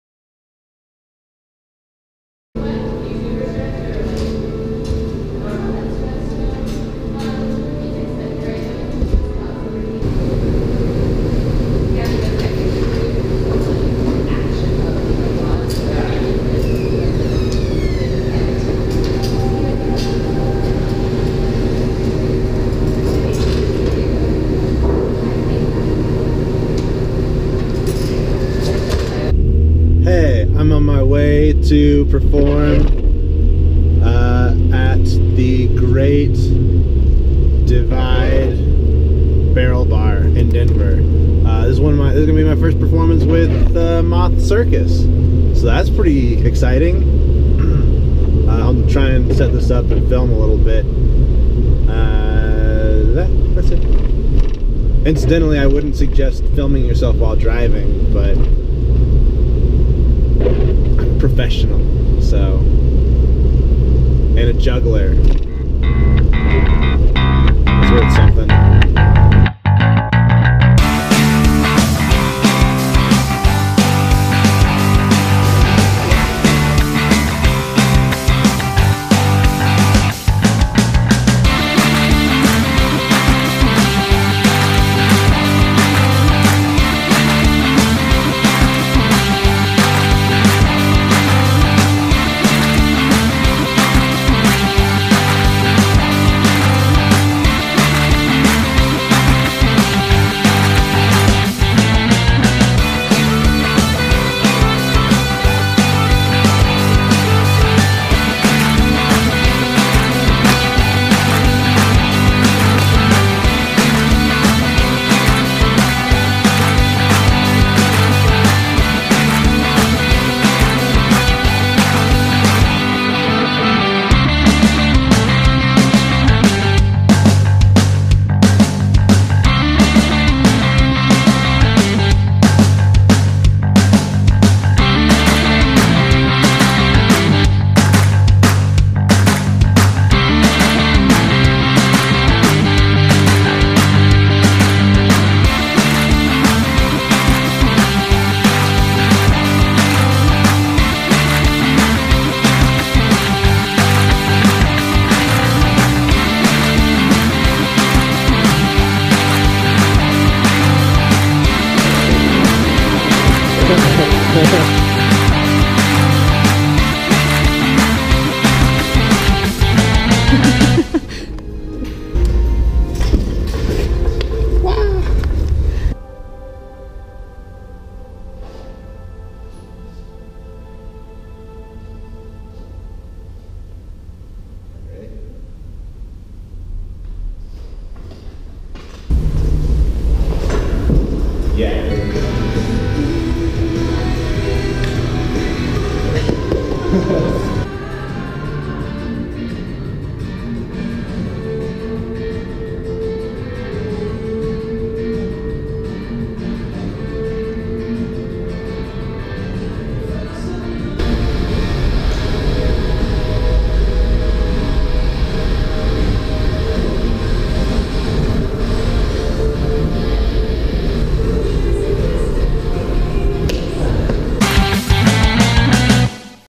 Ep. 4: Pole, Giant Orange Rhino, Duo Pole
This is but a small slice of what happened this week. I trained on the Chinese Pole, I performed at the RiNo Social at Great Divide Barrel Bar, and then Keilani and I worked on our Duo Pole act! I practically live on I-25 now.